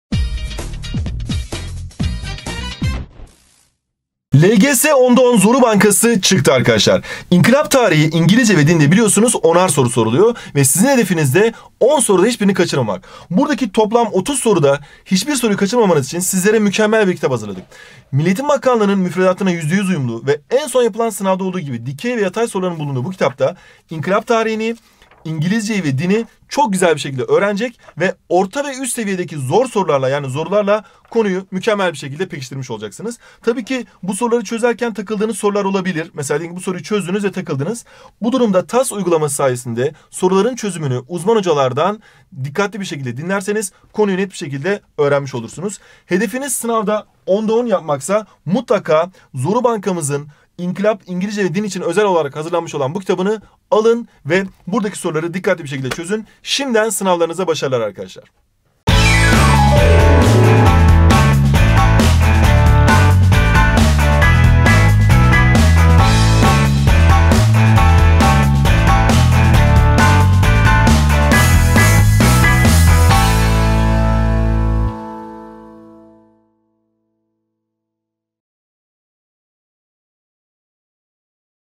8.Sınıf 10'da 10 Zoru Bankası ÇIKTI!
8.Sınıf 10'da 10 Zoru Bankası ÇIKTI! 8.Sınıf 10'da 10 Zoru Bankası, yenilenen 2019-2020 müfredatına %100 uyumlu. Kazanımları birebir test eden temel sorular, yeni nesil MEB tarzı ve gerçek yaşam durumlarına yönelik zoruları kapsıyor 🤓 Detaylı incelemek ve sahip olmak için hemen buraya tıklayabilirsin